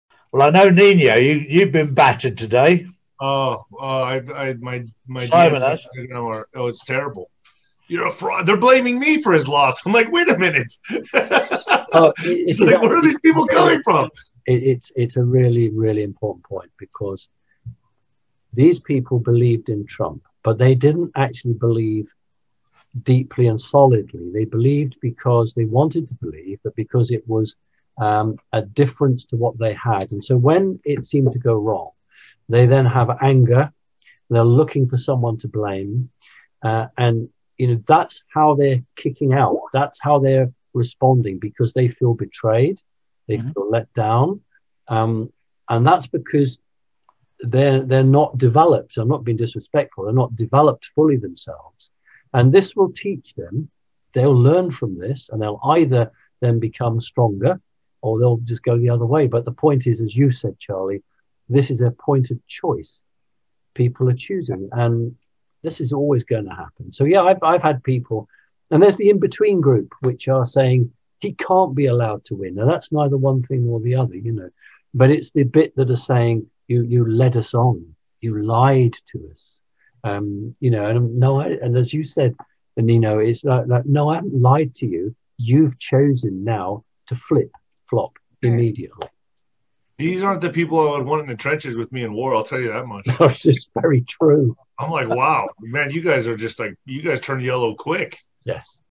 SIMON PARKES EXPOSED AGAIN - LET DOWN BY HIM? YOU ARE UNDER DEVELOPED SAYS SIMON
THIS IS WHAT SIMON PARKES THINKS OF YOU IF YOU FEEL LET DOWN OR LIED TO IN ANY WAY, IT IS OBVIOUSLY YOUR OWN FAULTS AND YOU ARE UNDER DEVELOPED
THE ABSOLUTE CHEEK OF THE MAN